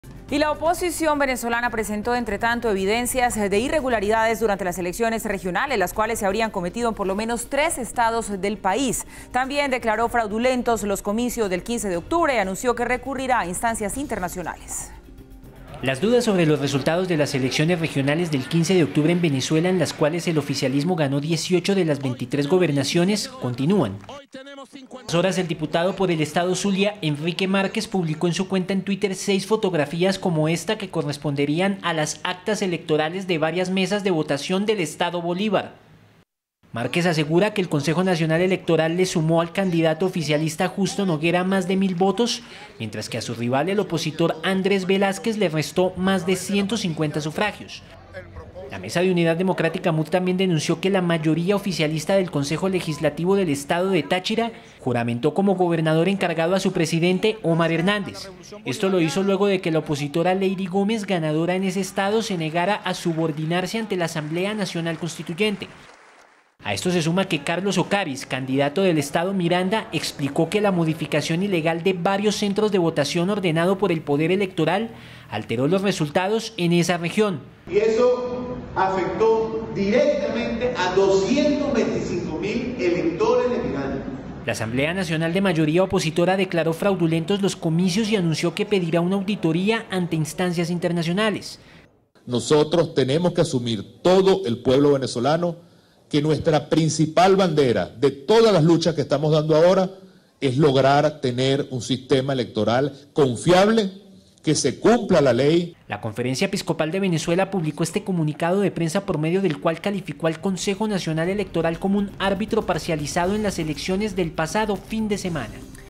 RED+ | Oposición denunció intentos de fraude electoral en tres estados de Venezuela
La oposición venezolana presentó evidencias de irregularidades durante las elecciones regionales, las cuales se habrían cometido en por lo menos tres estados del país, también declaró fraudulentos los comicios del 15 de octubre y anunció que recurrirá a instancias internacionales